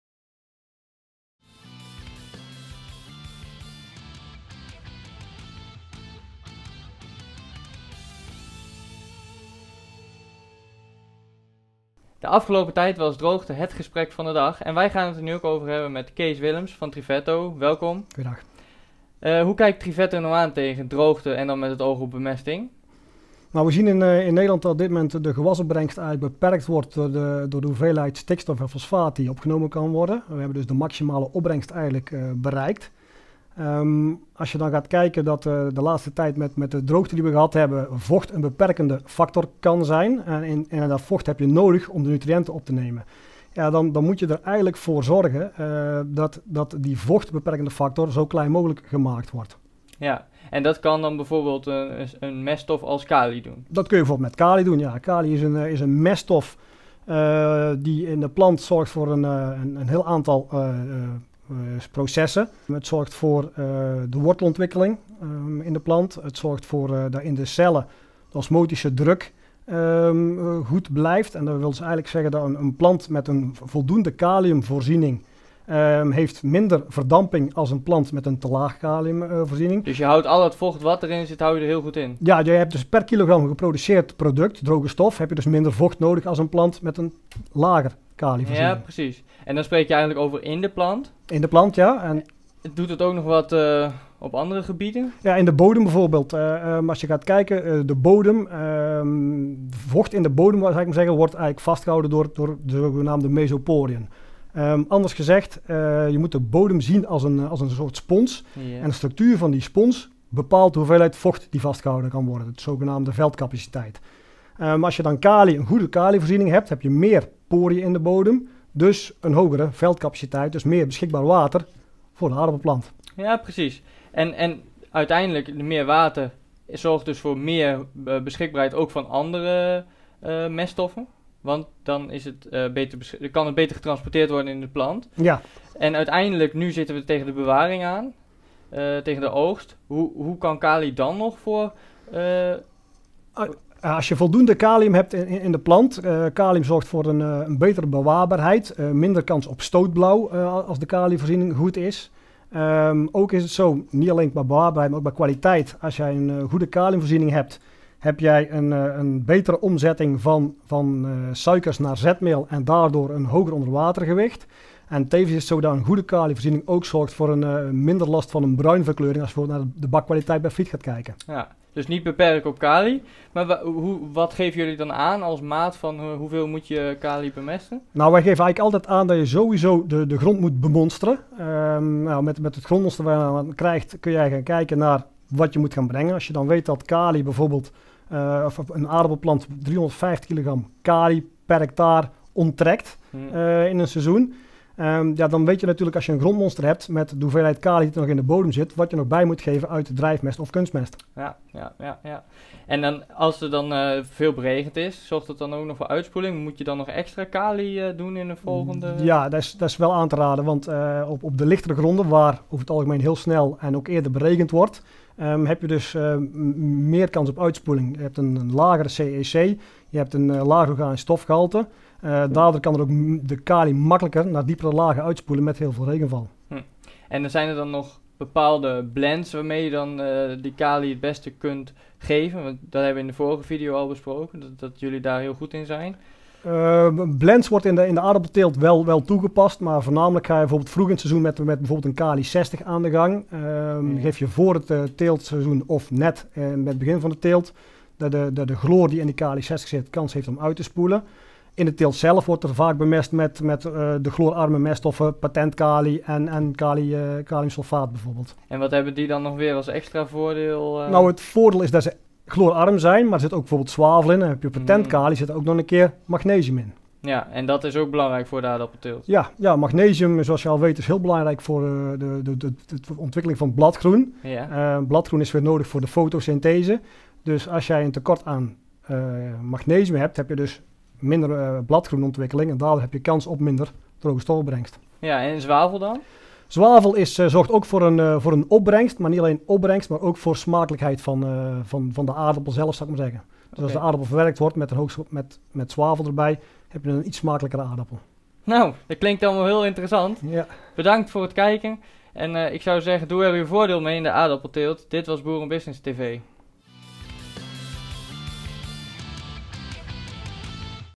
BB TV: Gewastour 2018 - 'Kaliumvoorziening belangrijk tijdens droogte'
"Het is belangrijk dat de kaliumvoorziening op orde is, met name in droge periodes. Het is daarnaast niet alleen belangrijk voor betere wortelontwikkeling, maar ook voor het waterhoudend vermogen van de bodem", zo zegt Cees Willems van Triferto.